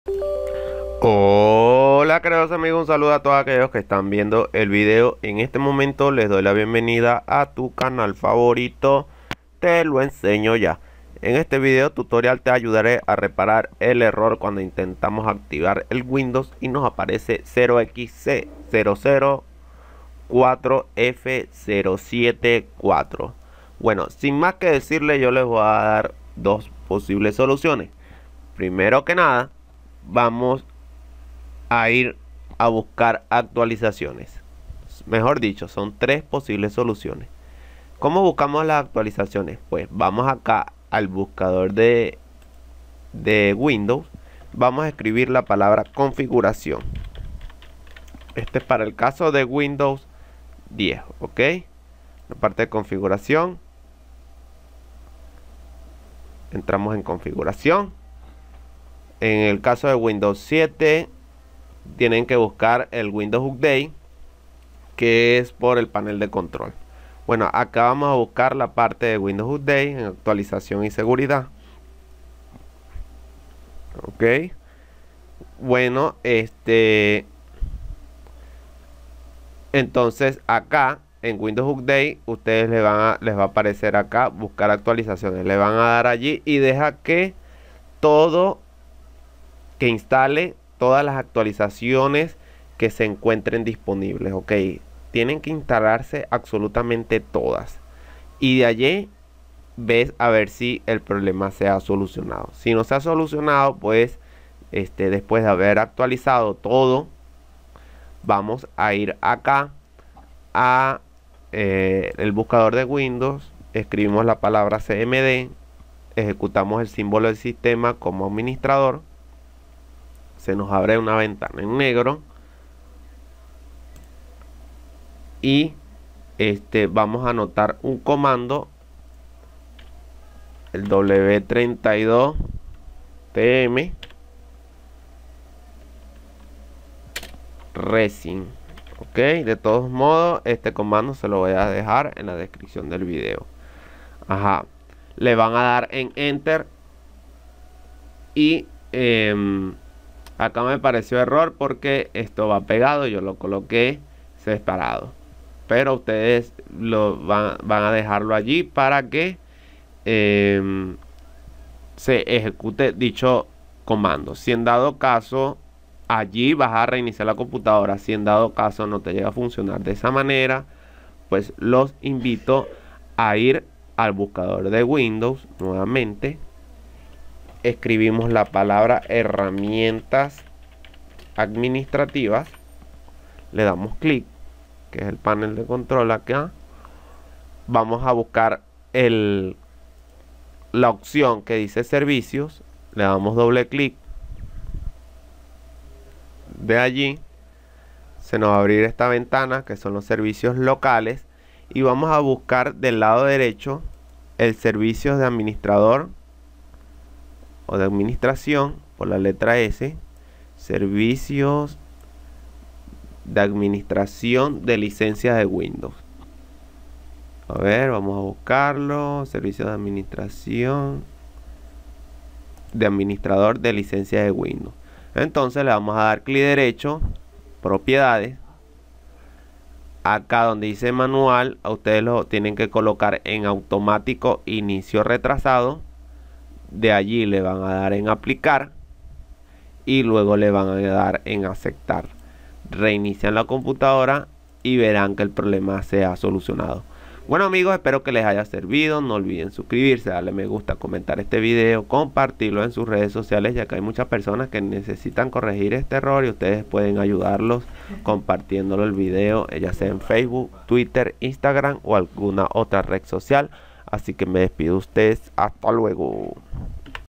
Error 0xC004F074 en Windows 10/8/7 I 2 SOLUCIONES 2024
Este video tutorial te dará a conocer la solución  error 0xC004F074 al actualizar windows 7/8/10 de una manera facil y sencilla